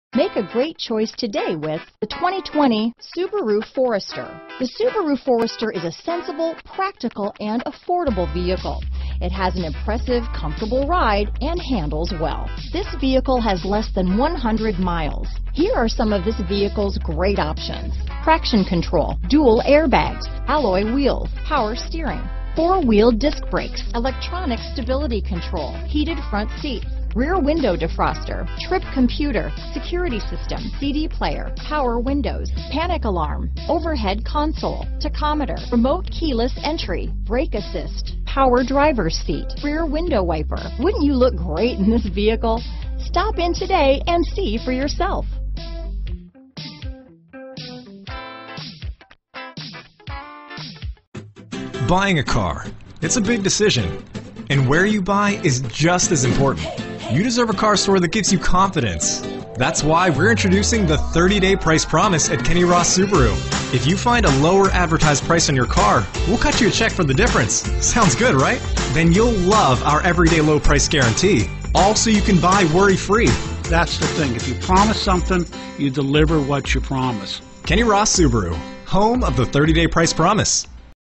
2020 Subaru Forester Pittsburgh, Greensburg, Monroeville, Irwin, North Huntingdon PA 20U3769
Crystal White Pearl New 2020 Subaru Forester available in North Huntingdon, Pennsylvania at Kenny Ross Subaru. Servicing the Pittsburgh, Greensburg, Monroeville, Irwin, PA area.

White Pearl 2020 Subaru Forester Premium 2.5L 4-Cylinder DOHC 16V VVT Lineartronic CVT AWD Well equipped with.brbrBlind Spot Detection/Rear Cross Traffic Alert +Keyless Access with Push-Button Start.